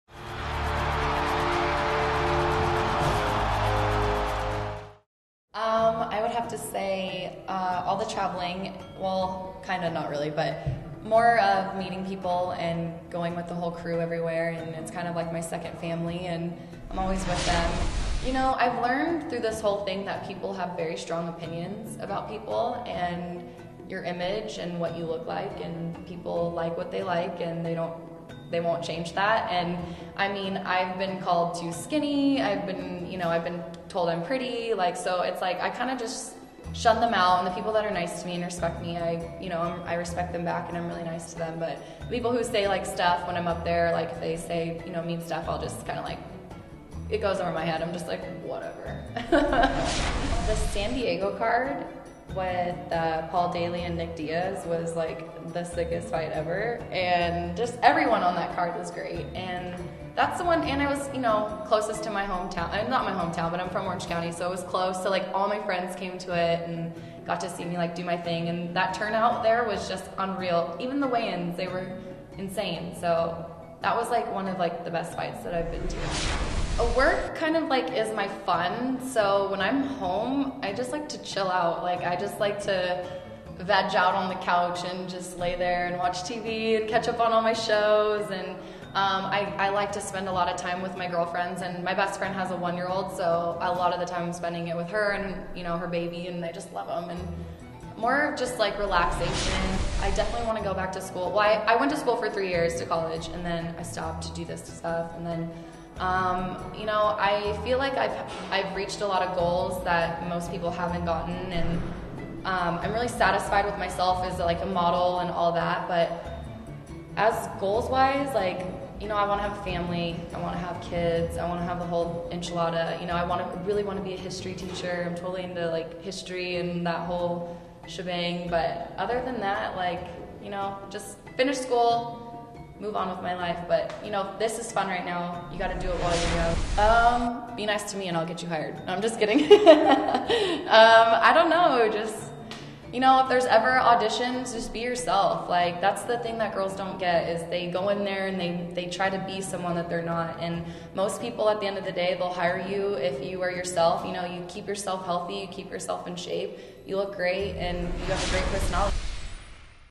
Kelli Hutcherson: Backstage Interview
Rockstar Ring Girl Kelli Hutcherson spoke to Strikeforce.com about life as the RGIC - Ring Girl In Charge. Check out the exclusive video, and be sure to vote for Kelli for Ring Girl of the Year at the 2011 World MMA Awards!